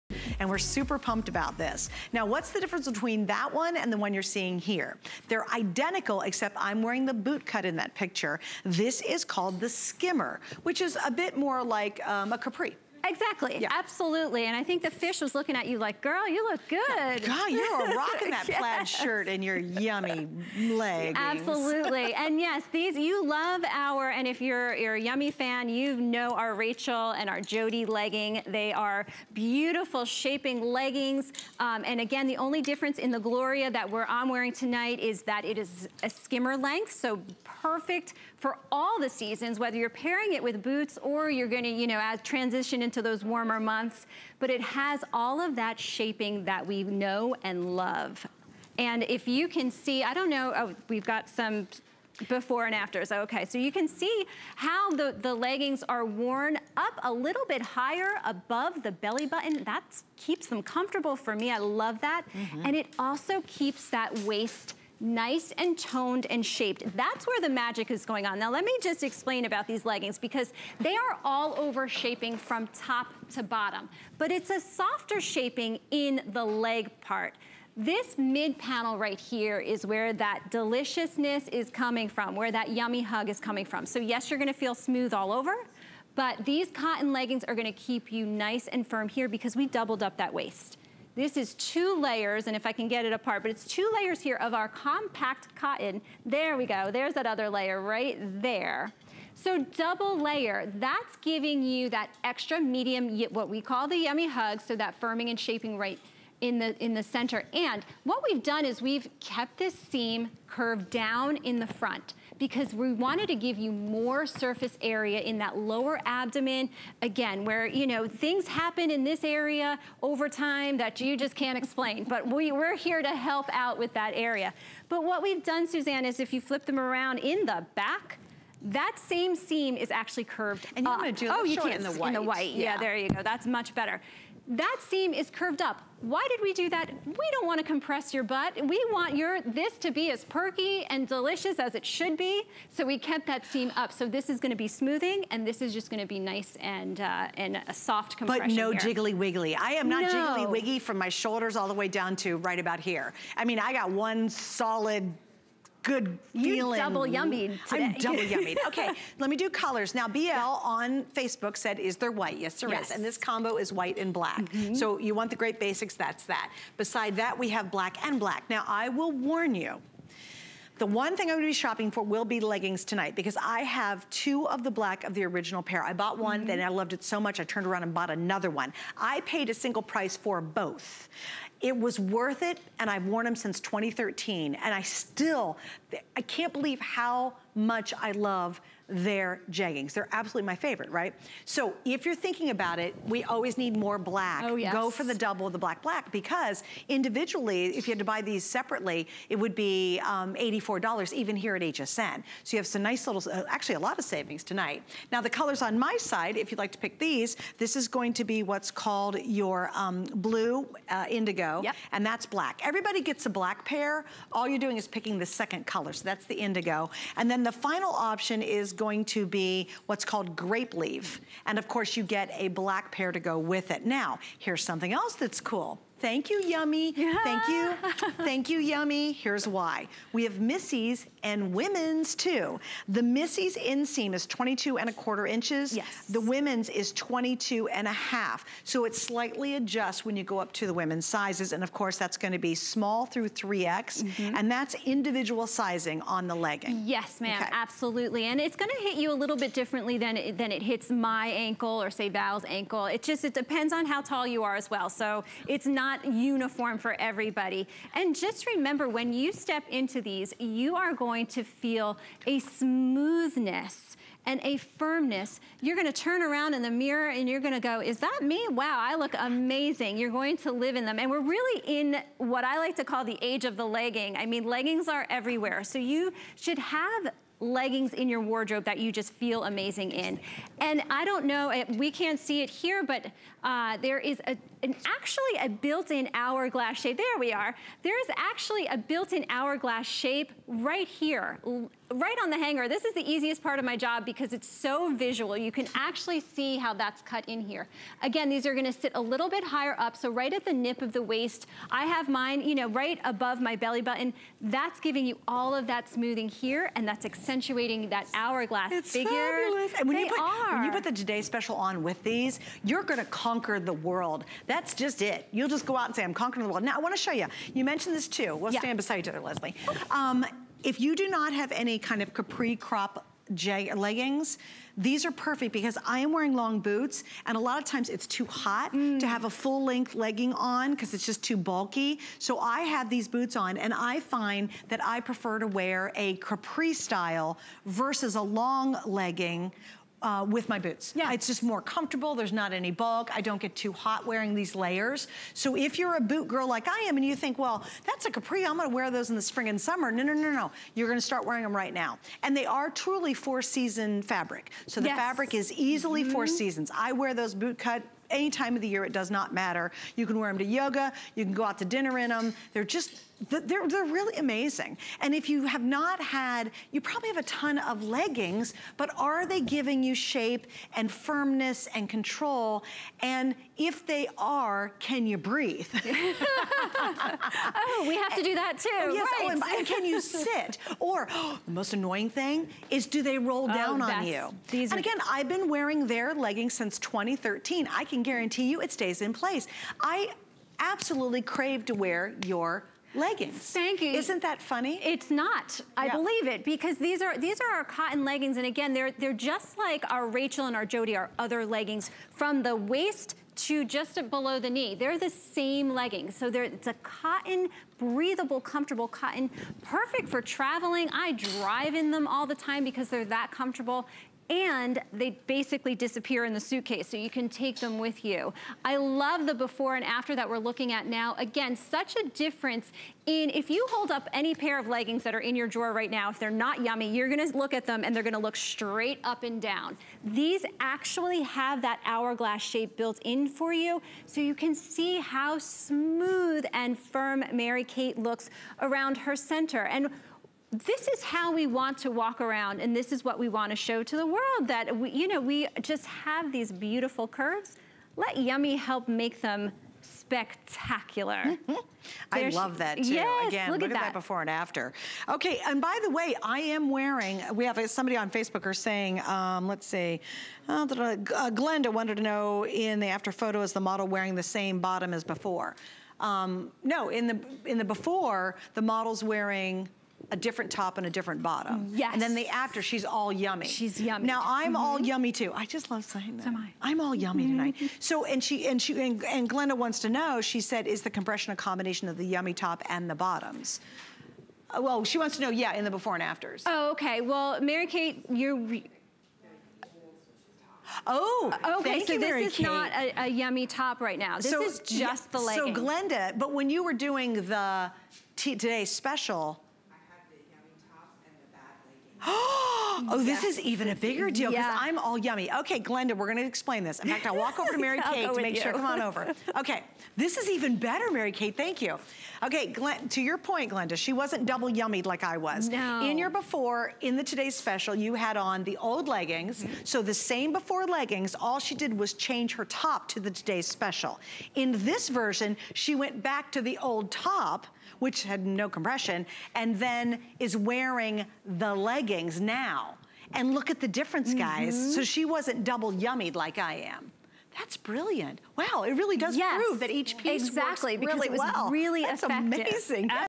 Yummie 2pack Gloria Skimmer Legging
Yummie's bestselling legging in a skimmer length, yay! Made from compact cotton, it provides allover smoothing with 360degree compression plus Yummie's...